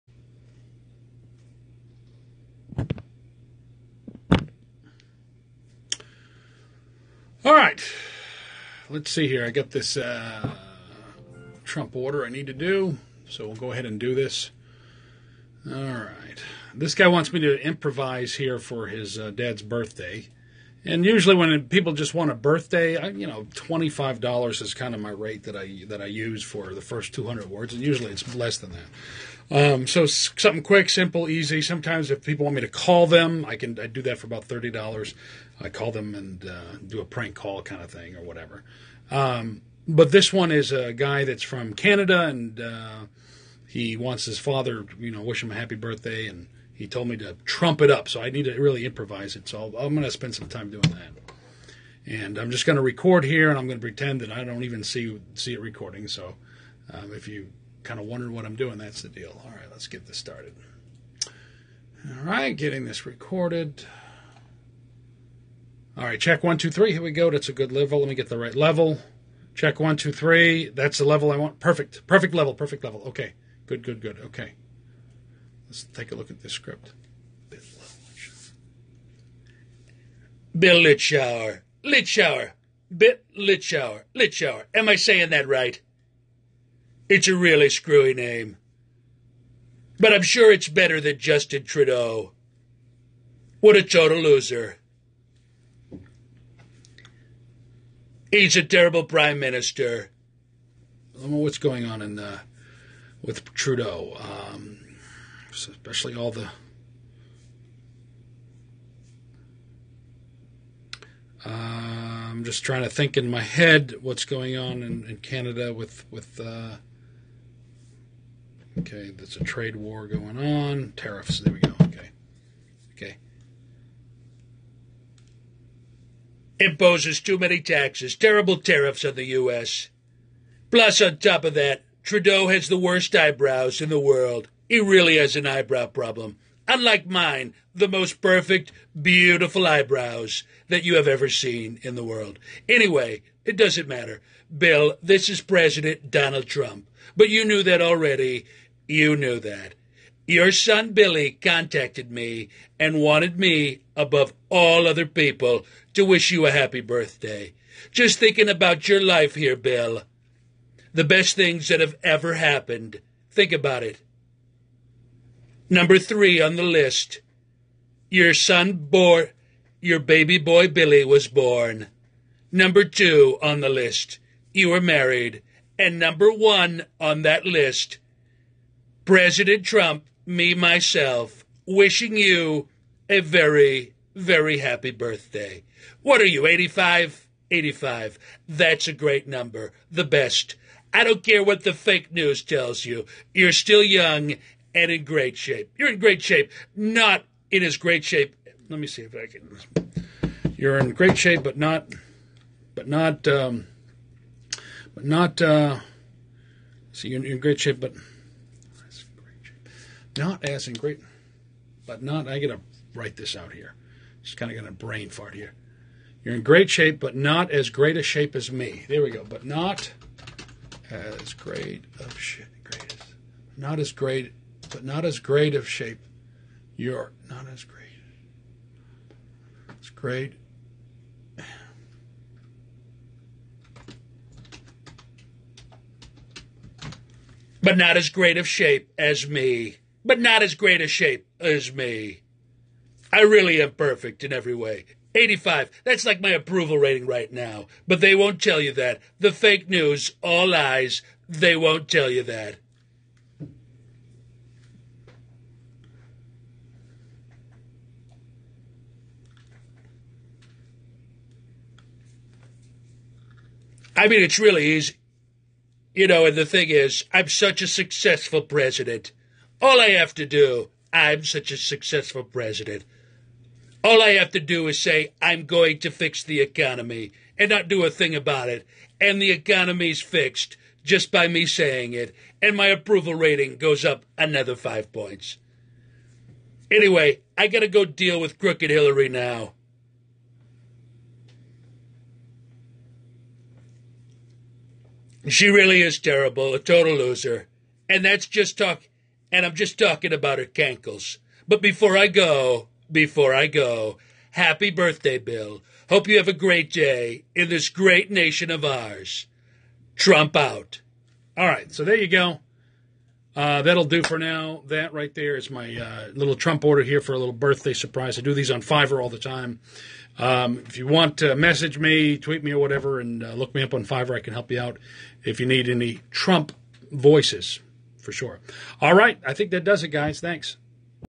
Trump voice on Fiverr happy birthday
rump rips Justin Trudeau's eyebrows. My Latest Trump impression on Fiverr